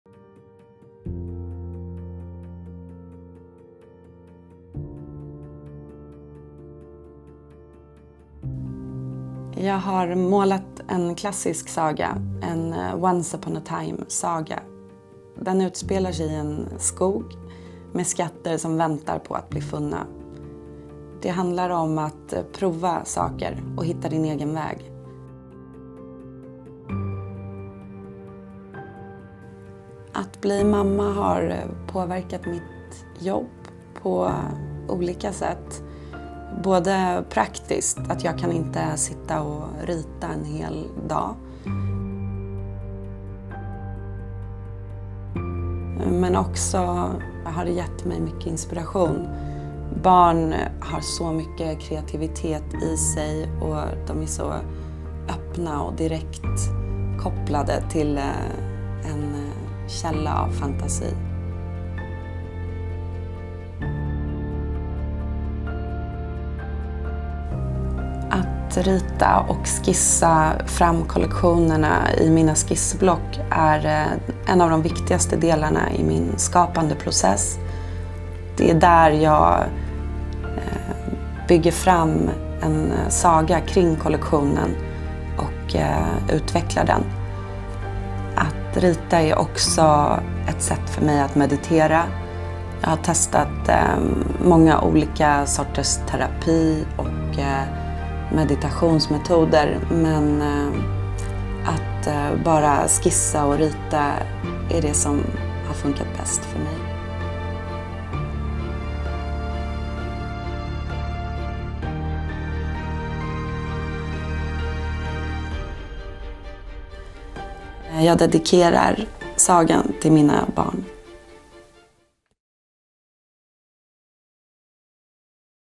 Jewellery designer Maria Nilsdotter reveals her secret talent in this project for Vogue Scandinavia
Maria Nilsdotter is best known for her eponymous jewellery line. But long before she began crafting her folklore-inspired creations, she would quietly sit and illustrate. We invited the Swedish designer to go back to her roots and paint an exclusive fairytale, dedicated to her children

Video by: Kristian Bengtsson

MB01ICAKVY5HE9C